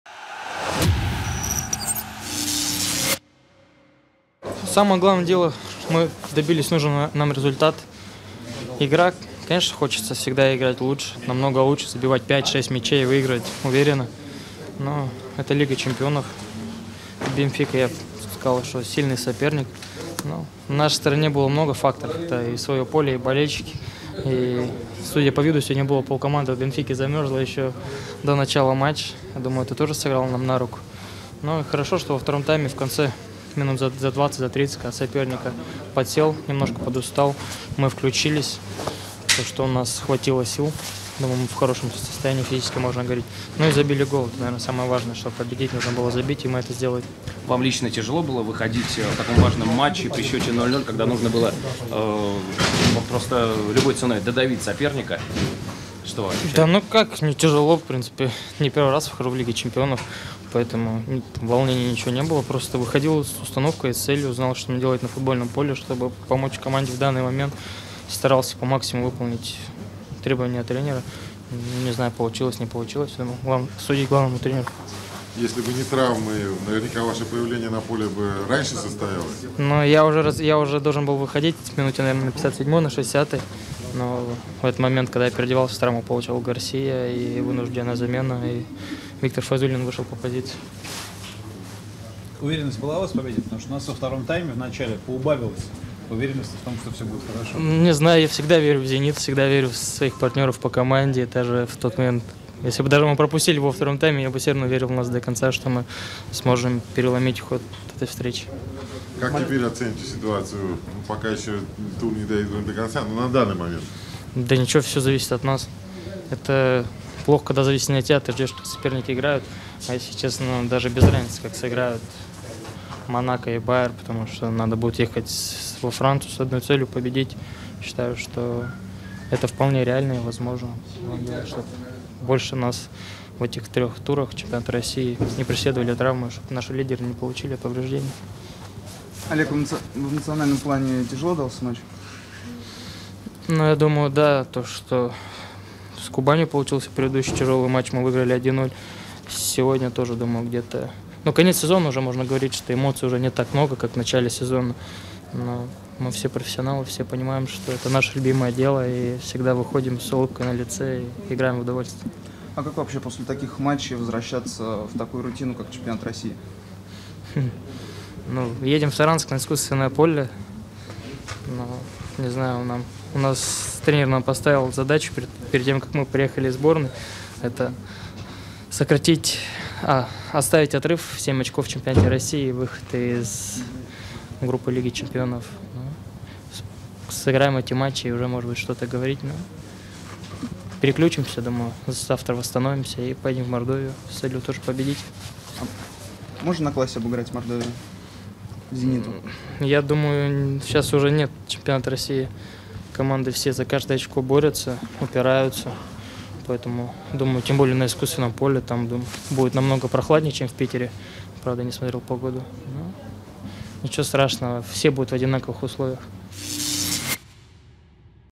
«Зенит-ТВ»: Шатов о победе над «Бенфикой» и нежелании играть в Лиге Европы
Интервью полузащитника ФК "Зенит" Олега Шатова после победы над "Бенфикой"